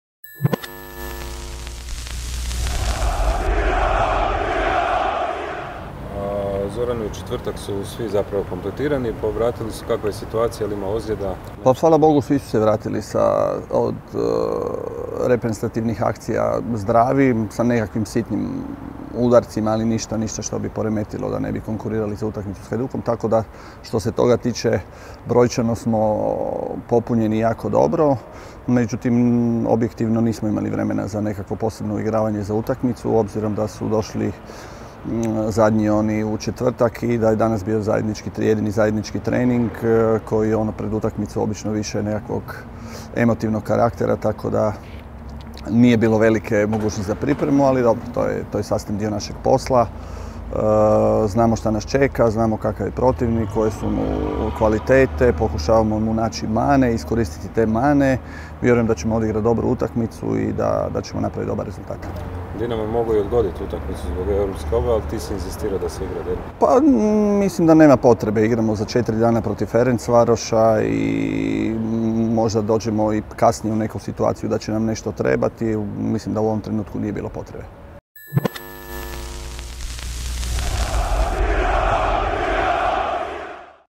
PRIJE UTAKMICE: Trener Zoran Mamić uoči puta u Split i utakmice protiv HNK Hajduk
Trener Zoran Mamić za naše kamere najavio je utakmicu protiv HNK Hajduk - što je rekao pogledajte u videu!